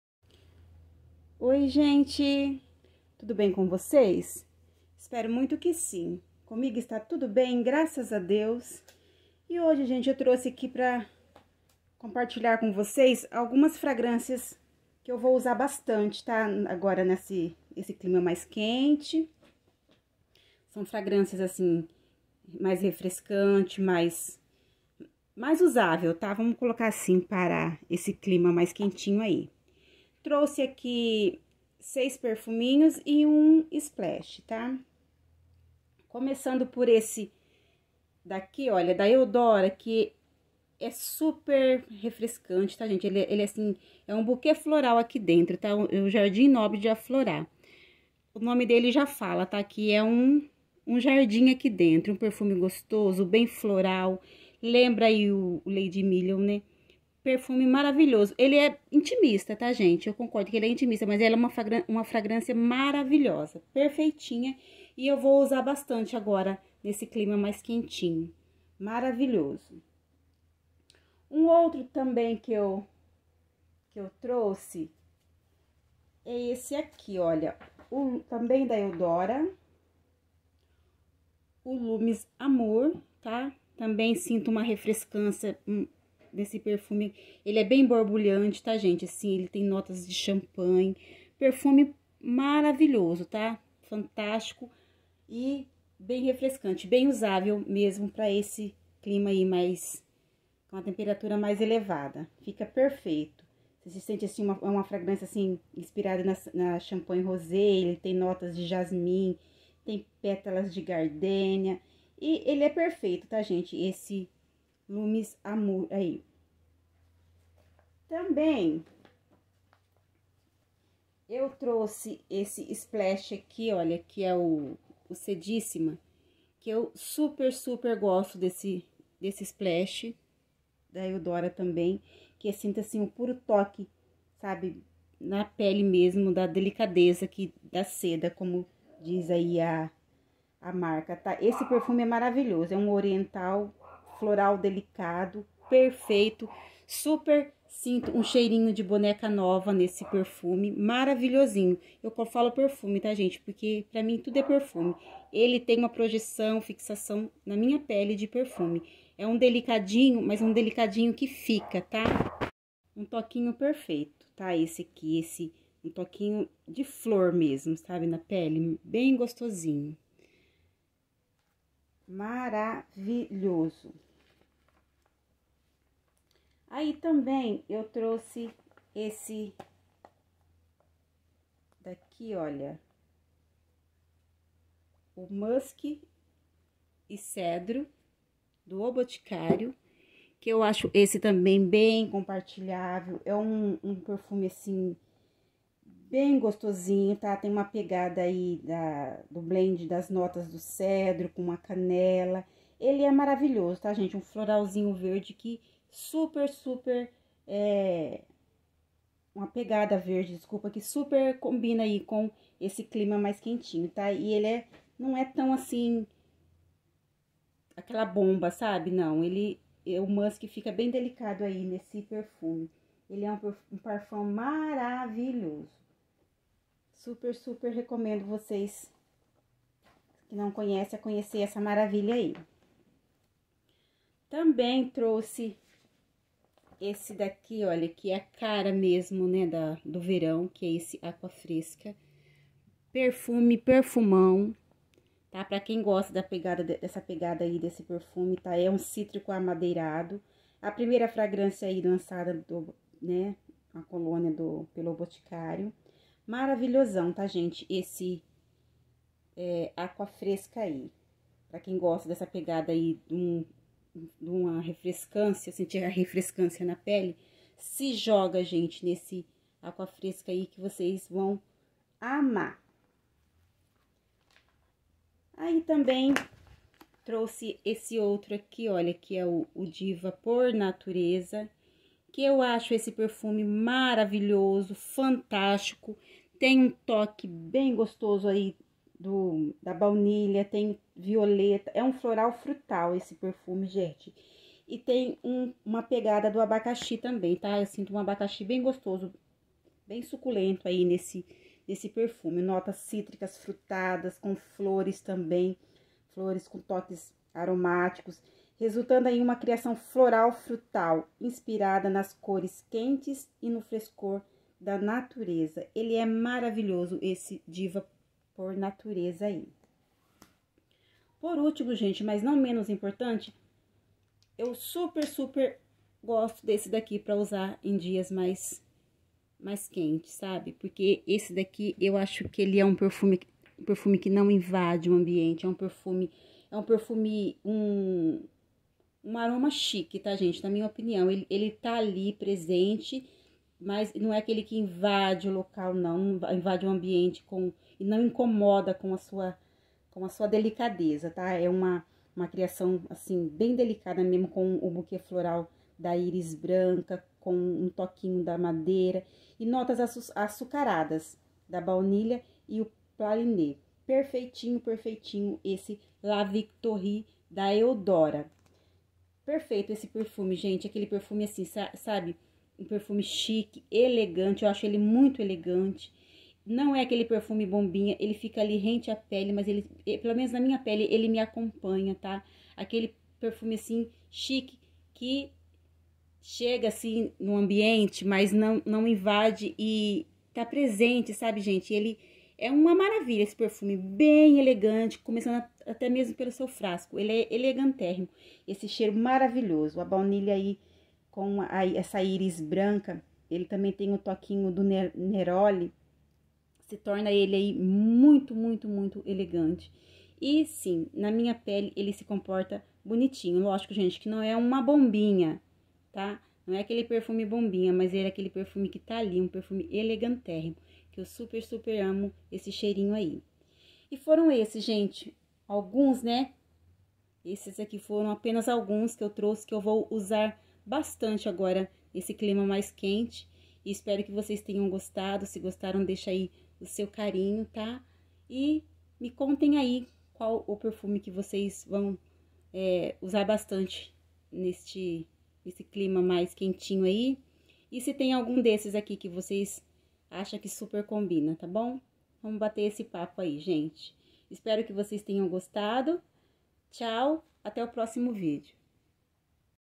Perfumes ideais para primavera/verão
Oi gente tudo bem com vocês espero muito que sim... trouxe algumas fragrâncias que vou usar muito nesse clima mais quente... primavera verão. são perfeitos refrescante ,florais ,florais frutais enfim perfeitos para se sentir cheirosa sem incomodar .espero que gostem 🌻